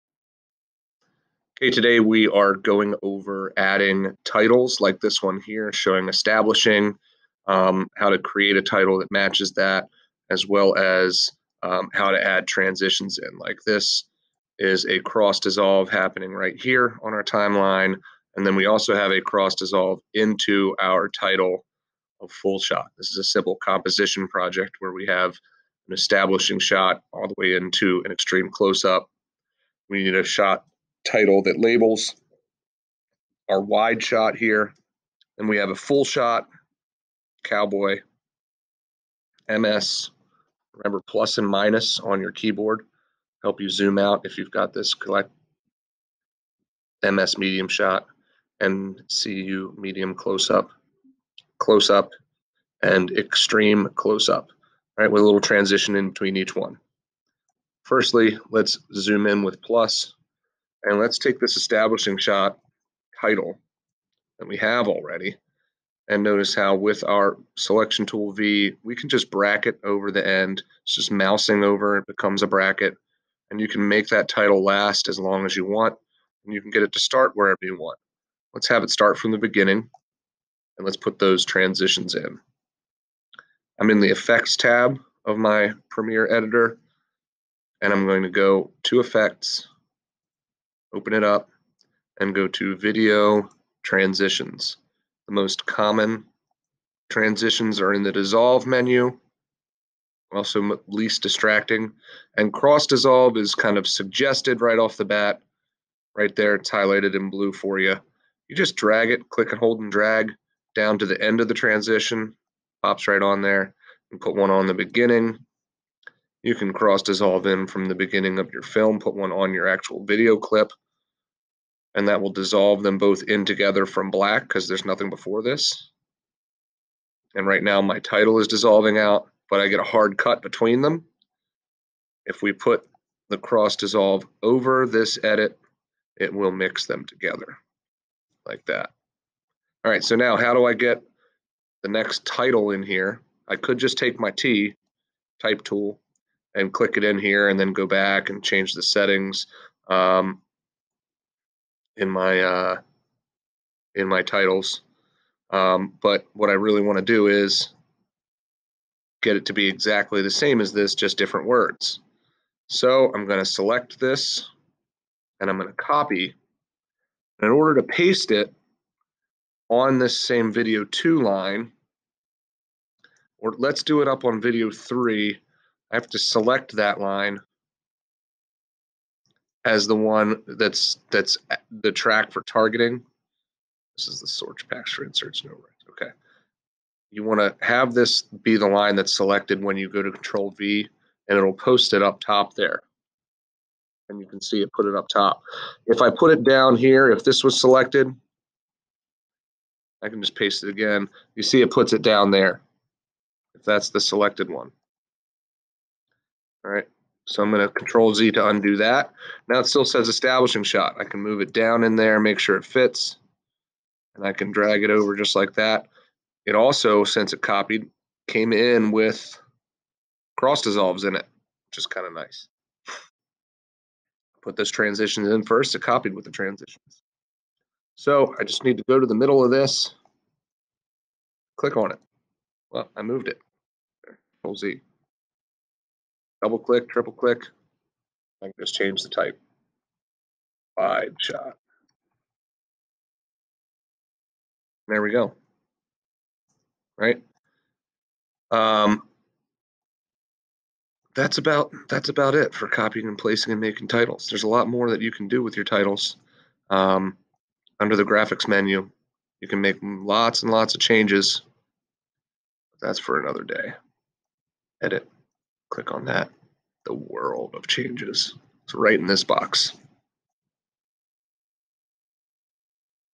Adding Simple Titles and Transitions to your Adobe Premiere Timeline
How to Add Simple transitions and text to your video.